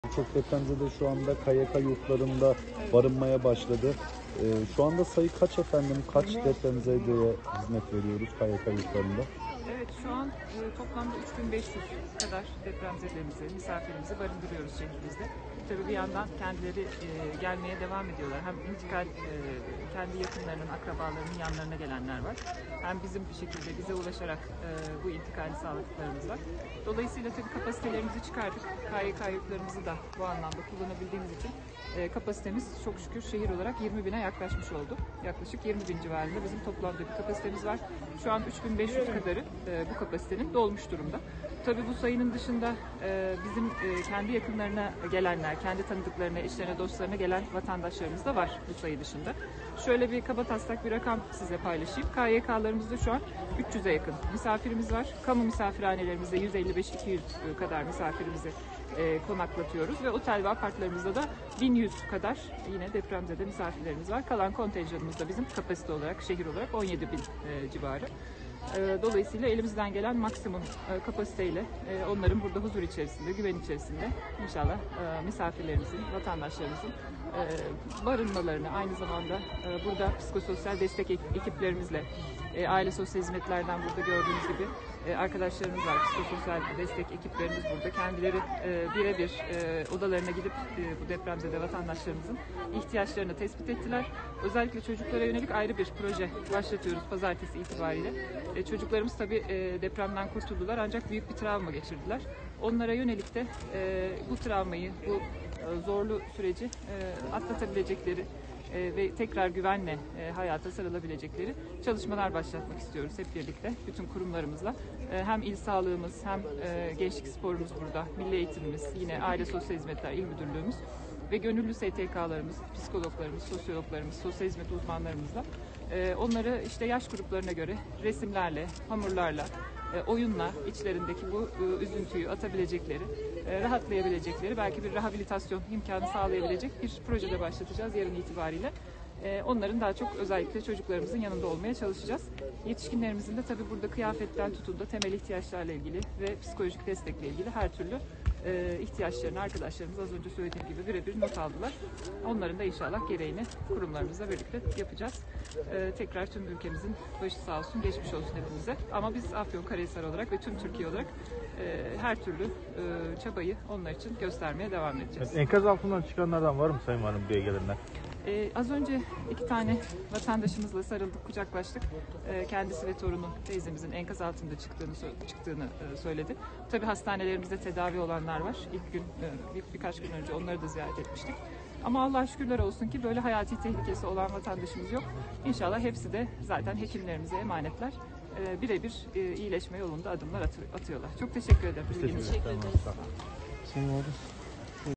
3 bin 500 depremzede Afyonkarahisar’a ulaştı
Vali Yiğitbaşı, toplam 3 bin 500 kişinin Afyonkarahisar’da barındığını belirterek toplam kapasitenin 20 bin kişi olduğunu ifade etti. Yiğitbaşı konuşmasının devamında, “Şu an toplamda 3 bin 500 kadar depremzedemizi, misafirimizi barındırıyoruz şehrimizde. Tabi bir yandan kendileri gelmeye devam ediyorlar” dedi.

Vali Kübra Güran Yiğitbaşı, Merkez üssü Kahramanmaraş olup 10 ili etkileyen depremin ardından şehirlerini bırakmak zorunda kalan ve Afyonkarahisar’a gelen depremzedelerle bir araya geldi. Kredi Yurtlar Kurumu Tınaztepe Erkek Öğrenci Yurdu'nda kalan vatandaşları ziyaret eden Yiğitbaşı, depremzedelerle yakından ilgilendi. Ziyareti sonrası açıklamalarda bulunan Yiğitbaşı, toplam 3 bin 500 kişinin Afyonkarahisar’da barındığını belirterek toplam kapasitenin 20 bin kişi olduğunu ifade etti. Yiğitbaşı konuşmasının devamında, şunları söyledi:
ŞEHİR OLARAK KONTENJANIMIZ 17 BİN CİVARI
“Şu an toplamda 3 bin 500 kadar depremzedemizi, misafirimizi barındırıyoruz şehrimizde. Tabi bir yandan kendileri gelmeye devam ediyorlar. Kendi yakınlarının, akrabalarının yanlarına gelenler var, hem de bir şekilde bize ulaşarak bu intikali sağladıklarımız var. Dolayısıyla toplam kapasitelerimizi çıkardık. KYK Yurtlarımızı da bu anlamda kullanabildiğimiz için kapasitemiz şehir olarak 20 bine yaklaşmış oldu. Bu sayının dışında kendi yakınlarına gelenler, kendi eşlerine dostlarına gelen vatandaşlarımız da var. KYK Yurtlarımızda şu an 300’e yakın misafirimiz var. Kamu misafirhanelerimizde 150-200 misafirimizi konaklatıyoruz. Otel ve apartlarımızda da bin 100 kadar depremzede misafirlerimiz var. Kalan kontenjanımız da bizim şehir olarak 17 bin civarı.”